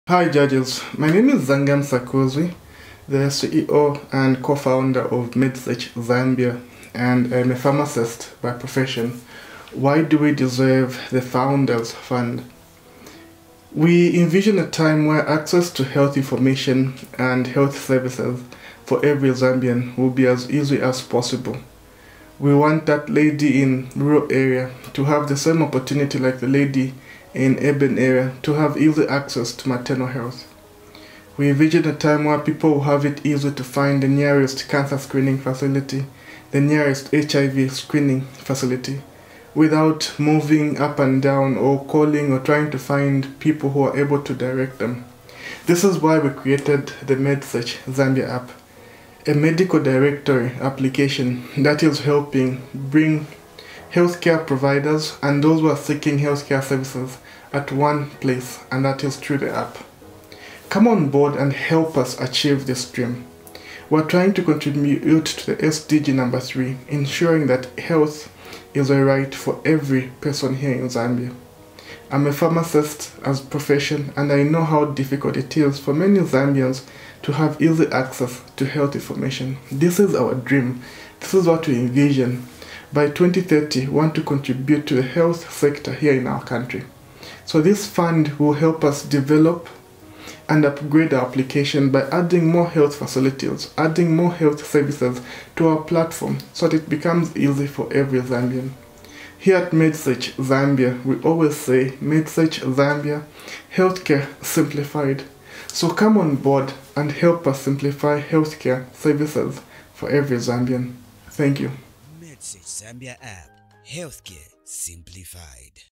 Google Black Founders Fund Pitch
have a watch as we give a short explanation why we need funders to come on board and help us make access to healthcare services as easy as possible for every Zambian